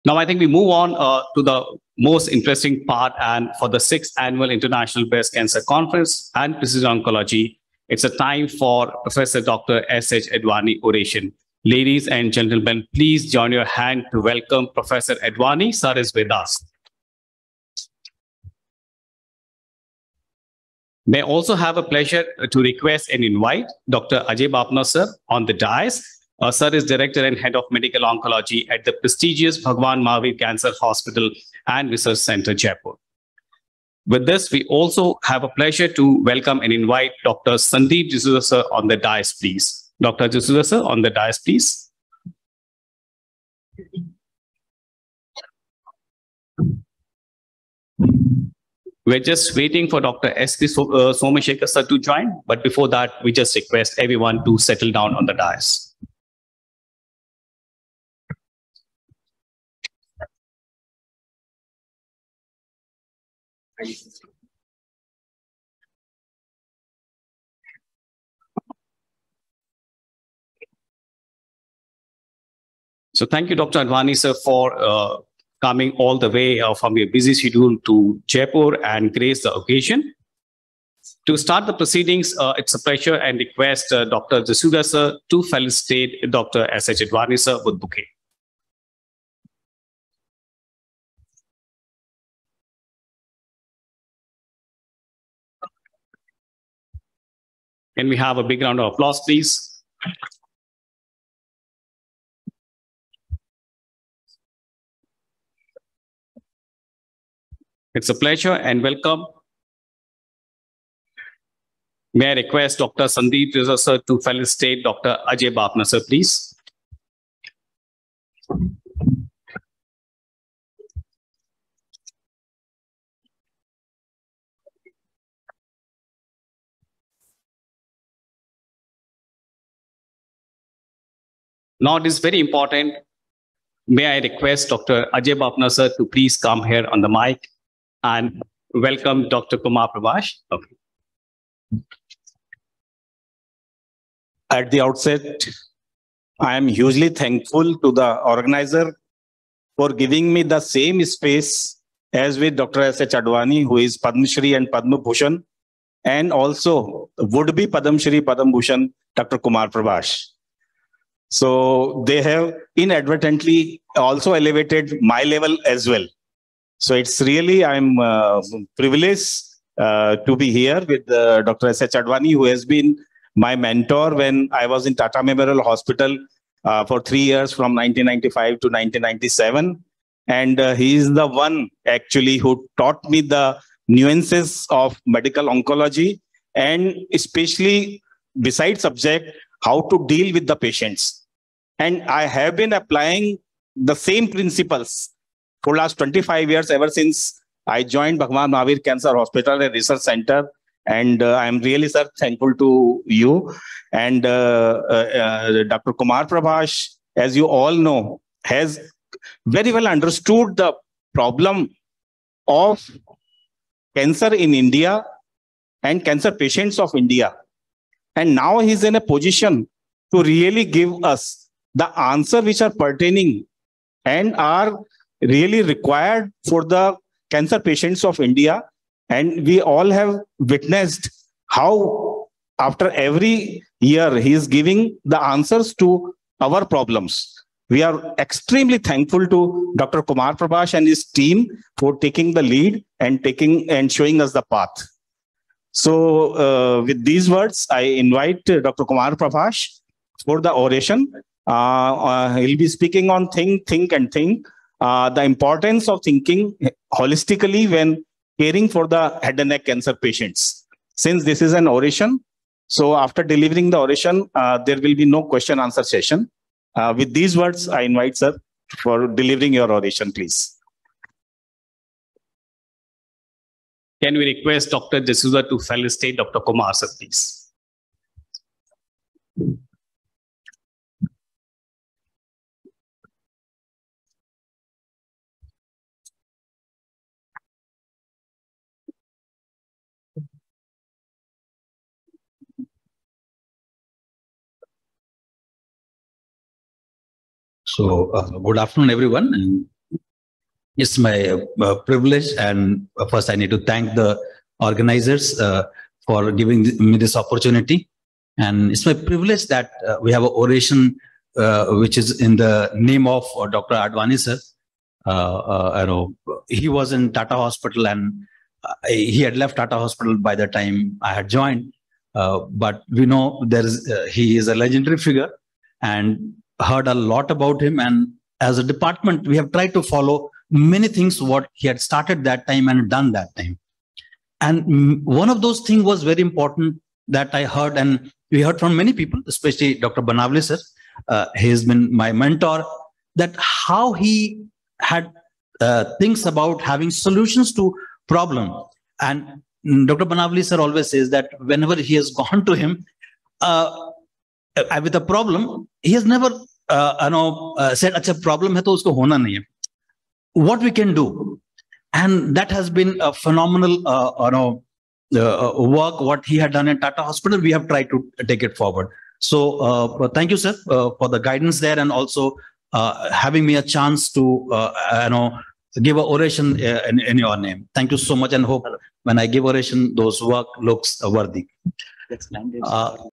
Welcome | Dr Suresh Advani
Cancer Research And Statistic Foundation organized a webinar "6th Annual International Breast Cancer Conference Precision Oncology 2022" with the support of "Institute of Pulmonology Medical Research & Development" & "Sunrise Oncology Centre".

Date: 19 Nov 2022 - Hall A - Precision Oncology 2022

Event Managed By:
Horizon Conference Management